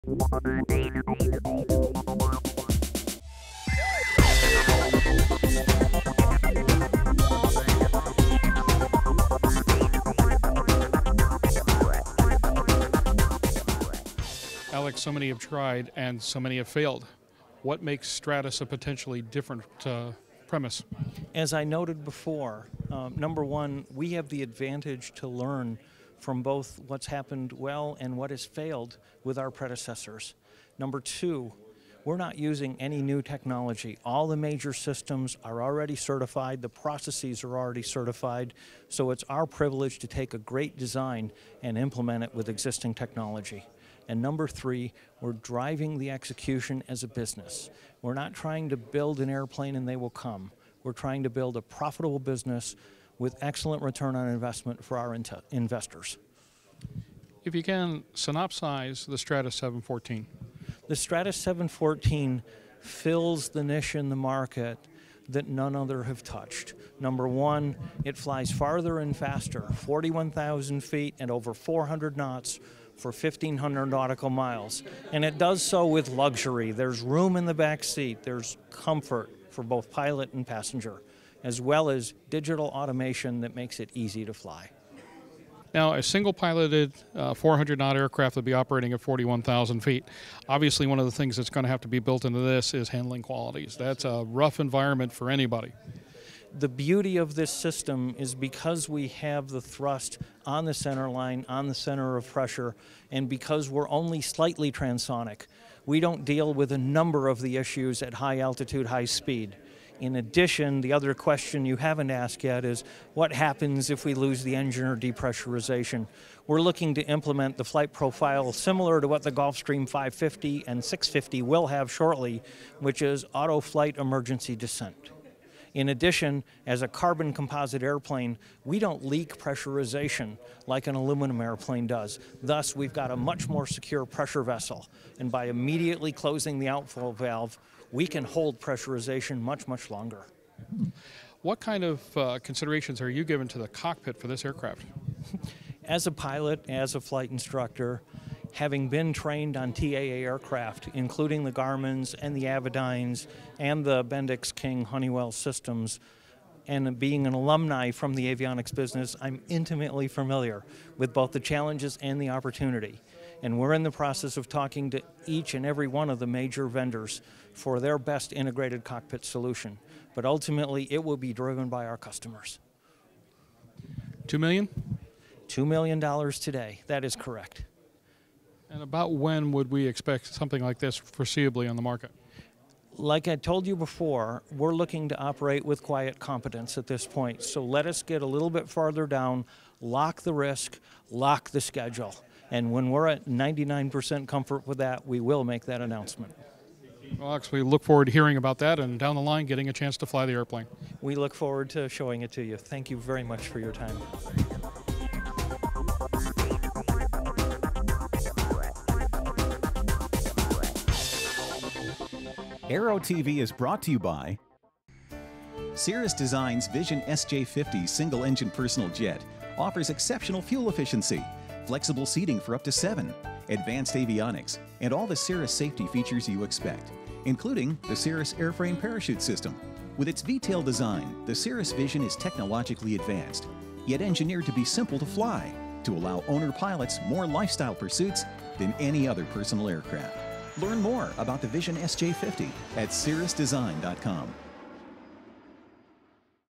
Aero-TV: Stratos 714 - A New Take On The Personal Jet
nother Company Has High Hopes For S/E Jet Market
There is perceptible process to be seen in the Stratos 714 jet program... which is a lot more than other such projects can say at this point. One of the first orders of business, though, at NBAA 2009 was to introduce a new boss. Stratos Aircraft introduced Alexander Craig as their new Chief Executive Officer during an NBAA news conference.
The Stratos 714 jet is designed to meet the needs of a growing number of performance-driven aviators 'who demand more speed range, and luxury in an owner-flown turbojet.' The 714 was first introduced last year and is the first VLPJ designed to bridge the performance gap between VLJ aircraft and full size business jets.
Stratos claims that the aircraft is a clean-sheet carbon composite design, optimized to achieve cruise speeds of more than 400 knots, with a 1500 nautical mile range (NBAA IFR reserves), with 4 people seated comfortably and ample baggage storage. Reportedly, a leading aerodynamics company has validated the primary performance targets of the Stratos 714 confirming their mission of 400 knots, 1500 miles, 4 passengers.